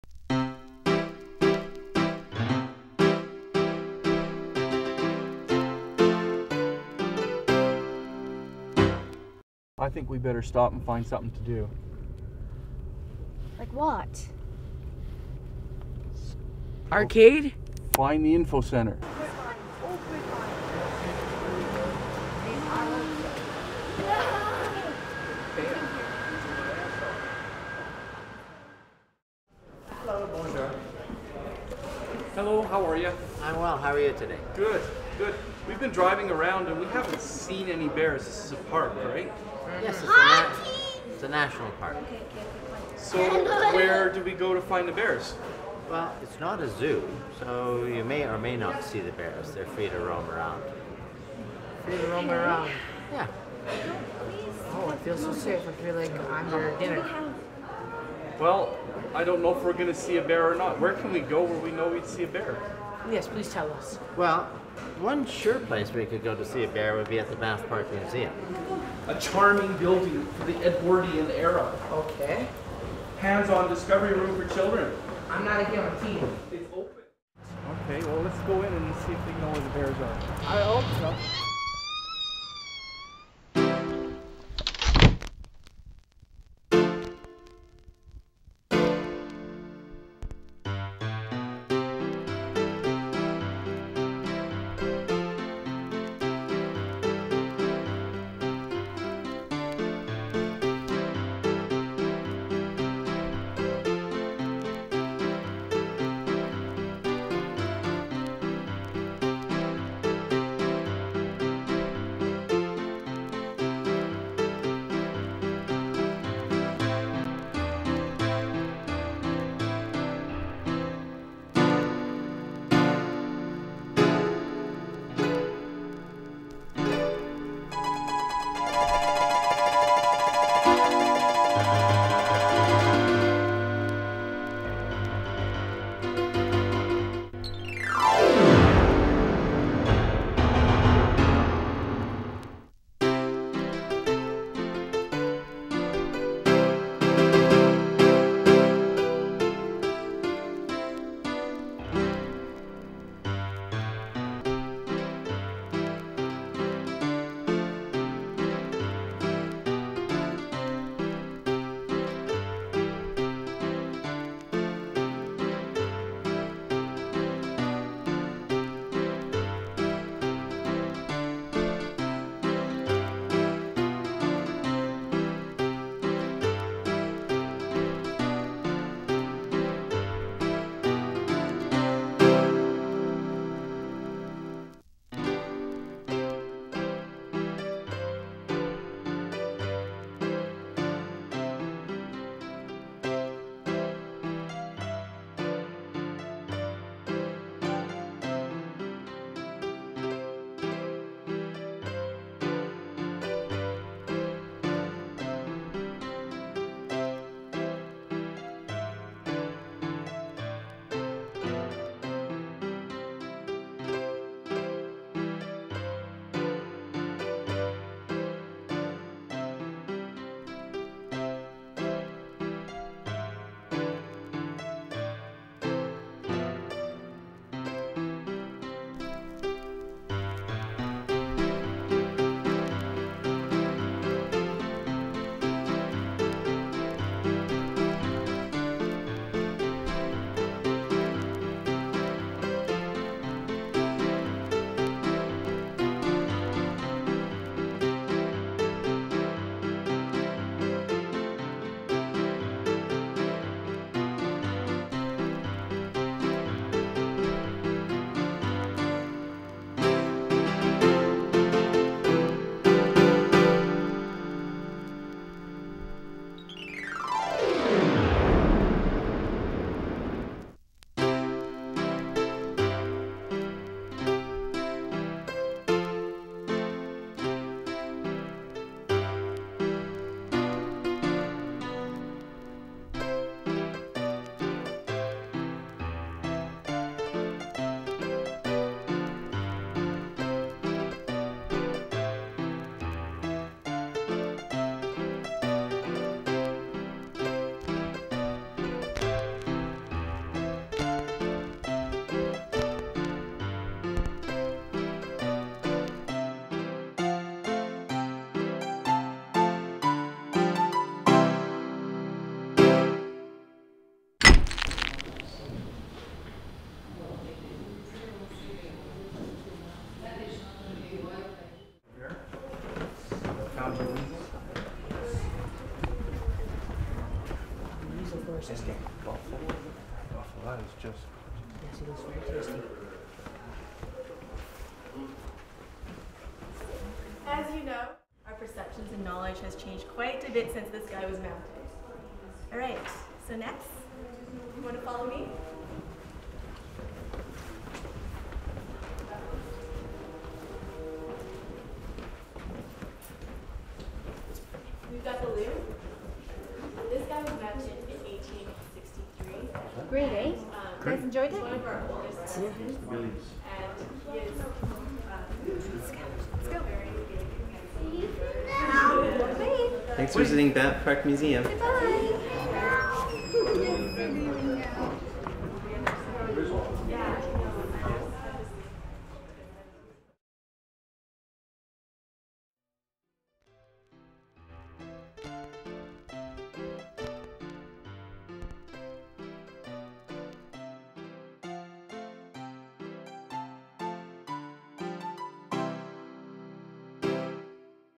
Banff Park Museum Video
A short film introducing the Banff Park Museum, a National Historic Site of Canada.
While searching for bears, a family discovers the Banff Park Museum. Upon entering the museum they are given a tour by Norman Sanson, the last curator of the museum. Since his retirement in 1932, much of the historical collection and original architecture has remained the same, making the museum itself an exhibit.